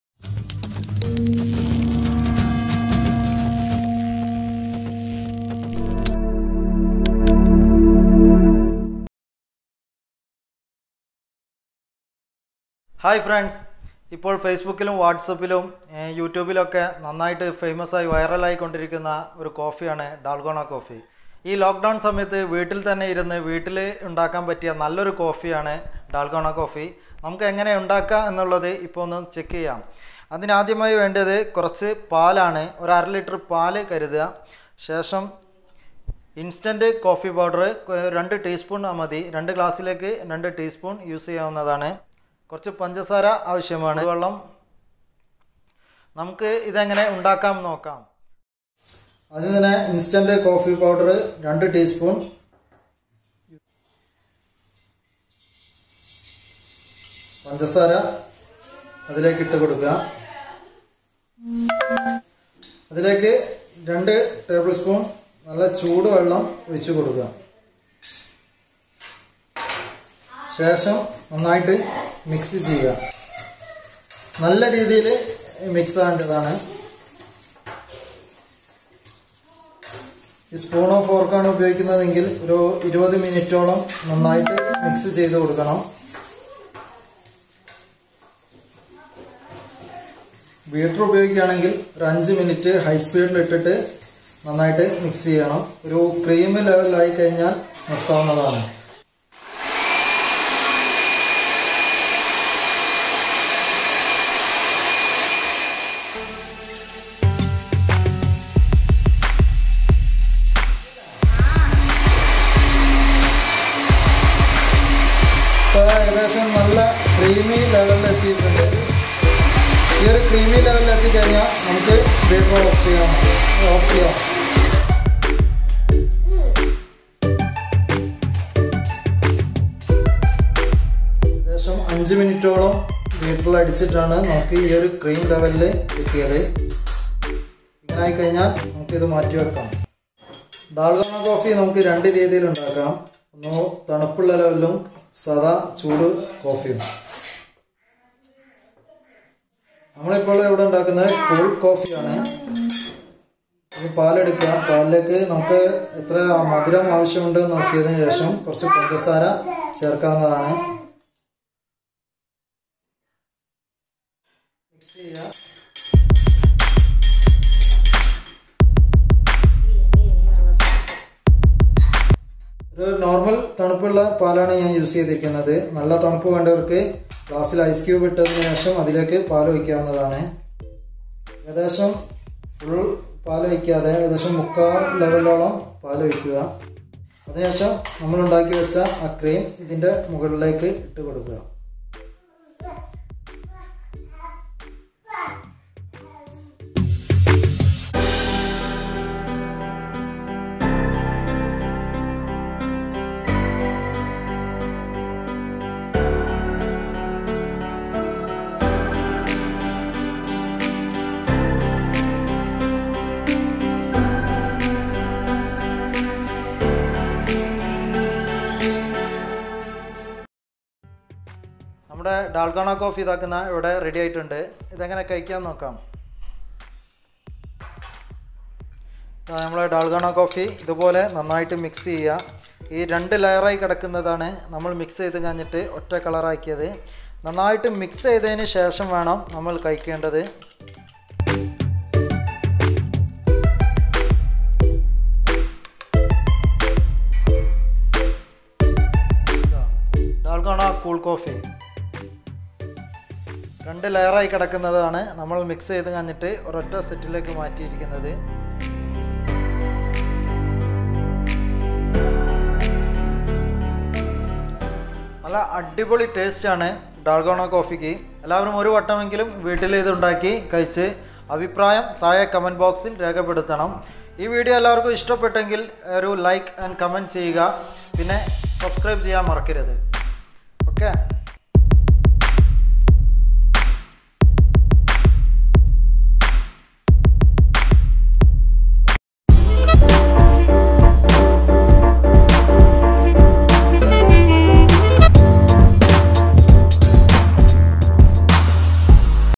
Dalgona Coffee Recipe - How To Make Dalgona coffee
Dalgona coffee is a beverage made by whipping equal proportions of instant coffee, sugar and hot water until it becomes creamy and then adding it to cold or hot milk. Occasionally, it is topped with coffee powder, cocoa, crumbled biscuits, or honey.It is not possible to make Dalgona coffee using ground coffee beans; instant coffee creates the dense and foamy topping and the reason for this has much to do with the drying process of the coffee granules.